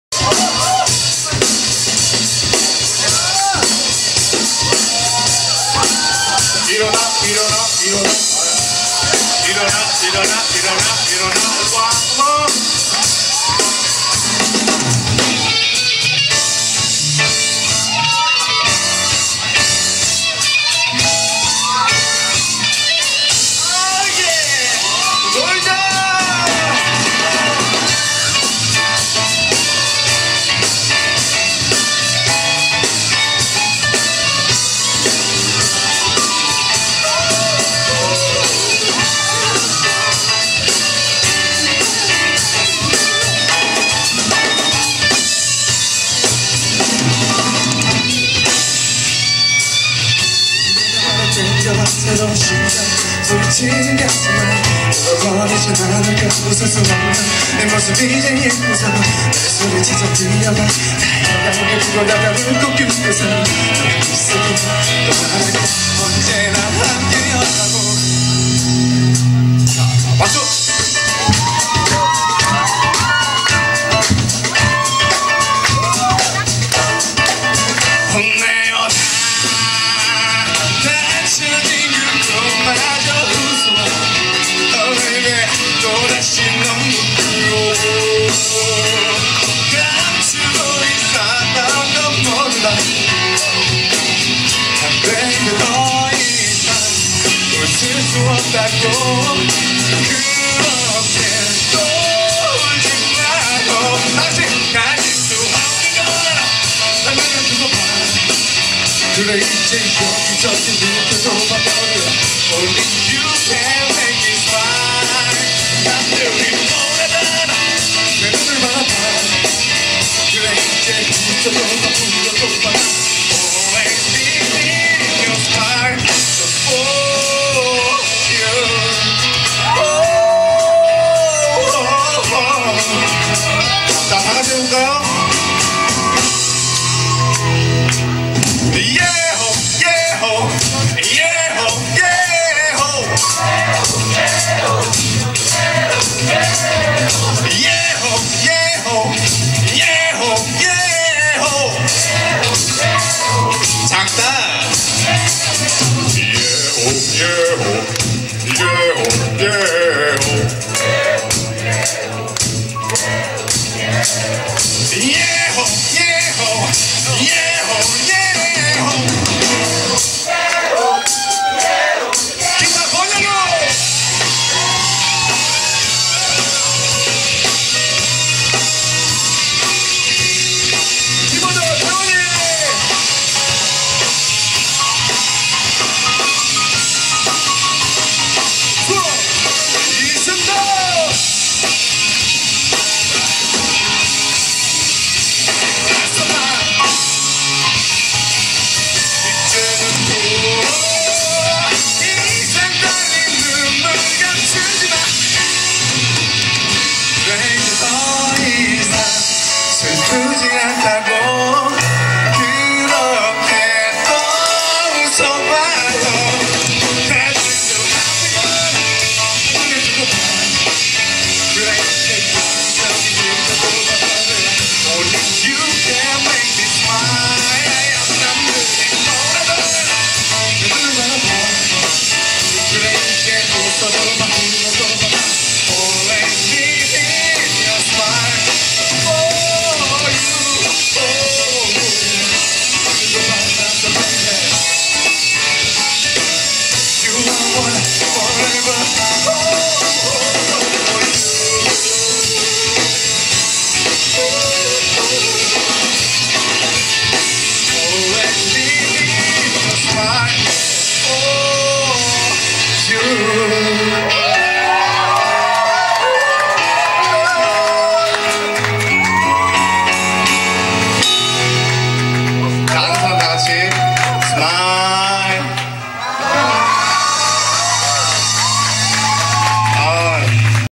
091227 타임레이블 쇼 - 원상욱밴드 1
공연이름 : 타임레이블 쇼
 공연기획 : 타임엔터테인먼트
 공연날짜 : 2009년 12월 27일 (일)
 공연밴드 : 원상욱밴드
 공연촬영 : ★싸이락★ - 락음악 커뮤니티
 촬영기종 : 삼성 HMX20C   외장마이크